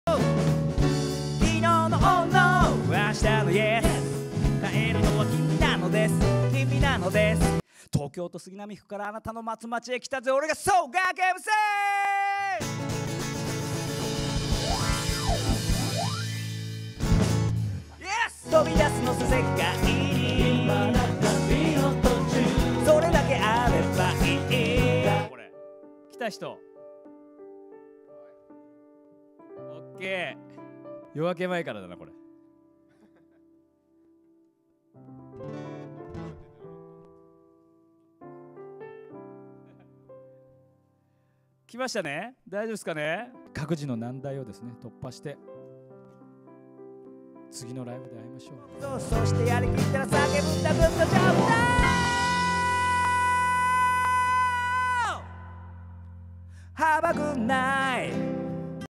GAKU-MC たちころLive 映像が到着！
GAKU-MC ONLINE LIVE 2020『立ち上がるために人は転ぶ』
11/29(日)配信のライブの模様が、12/2(水) 23:59までアーカイブ配信中！
チケットのご購入もまだ間に合います!!